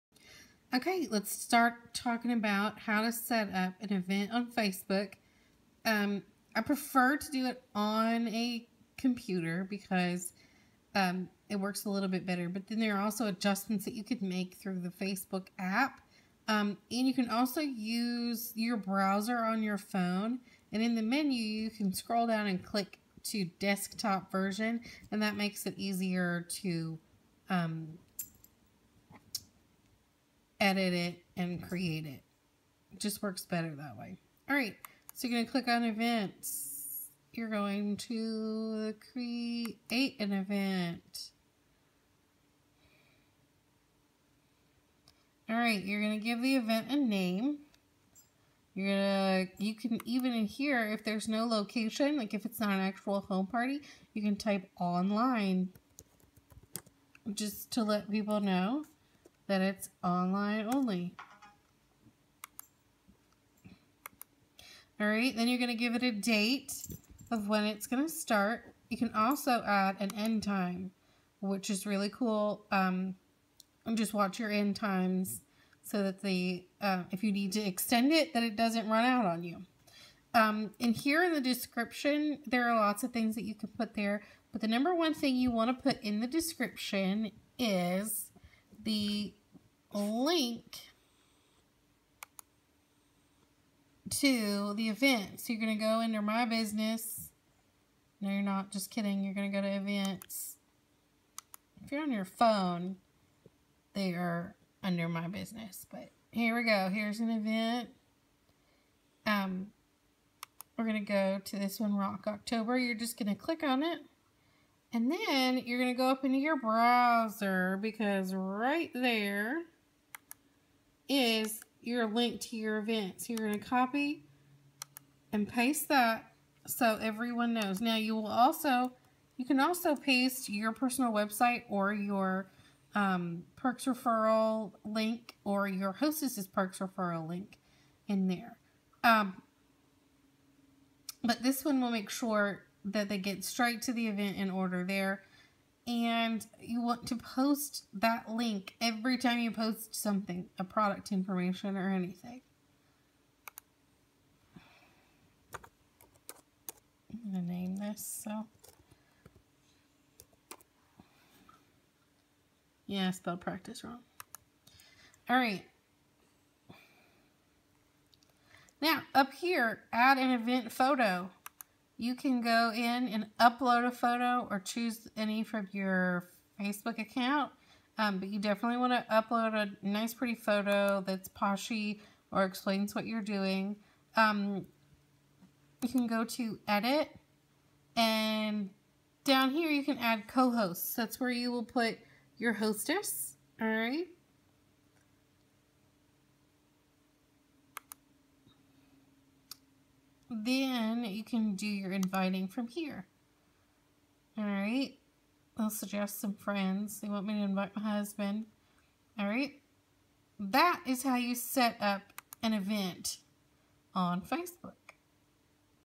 NEW! How to set up an event on Facebook!!!
Online, portable and home parties can benefit from setting up an event on Facebook! This training will help you learn how to do it.